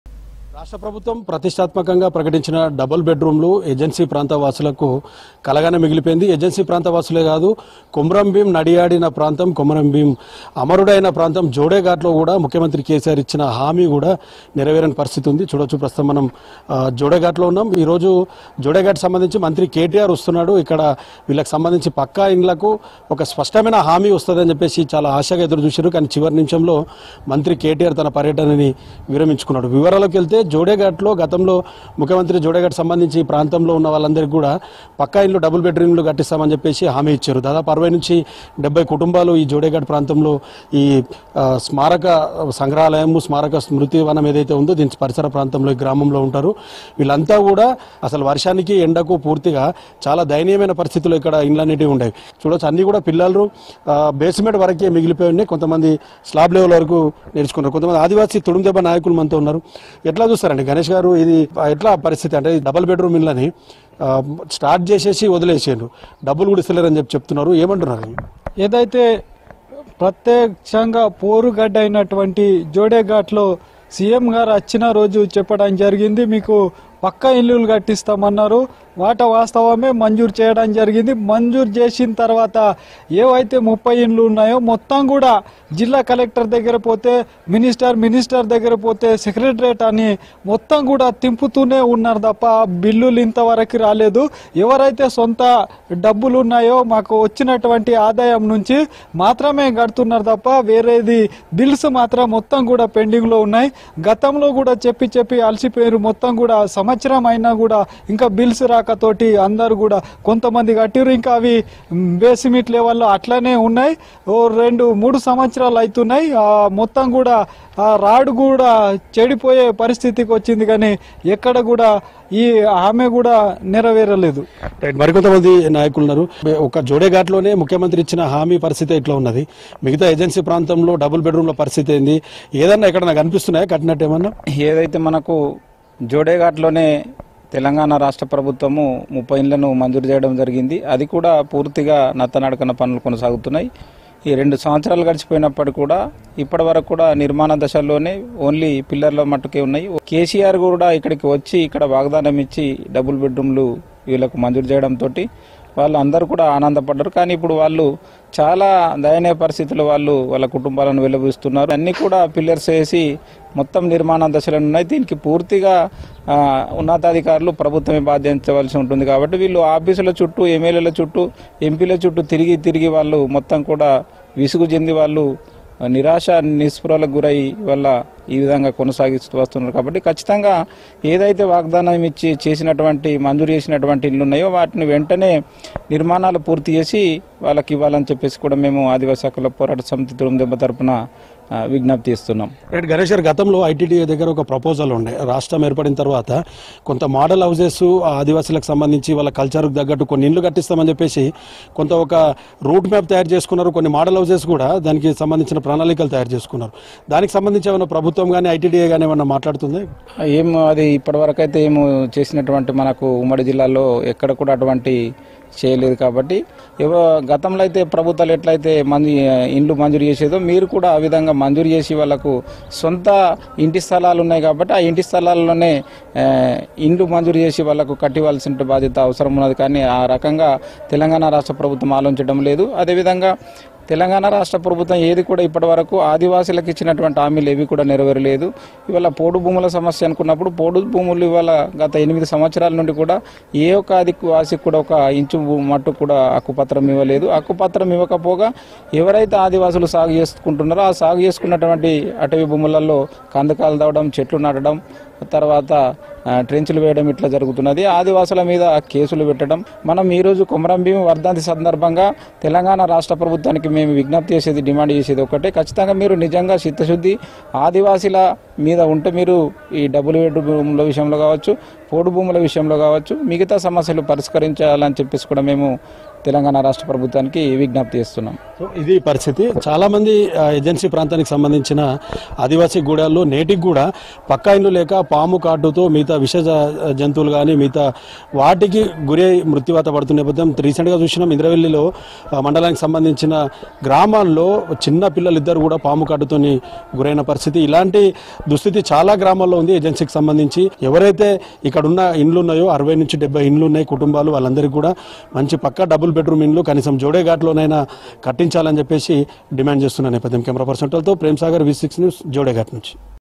Jodeghat Villagers Serious On State Govt Over Delay Of Double Bed Room Houses Construction Works |V6
Jodeghat Villagers Serious On State Govt Over Delay Of Double Bed Room Houses Construction Works | V6 News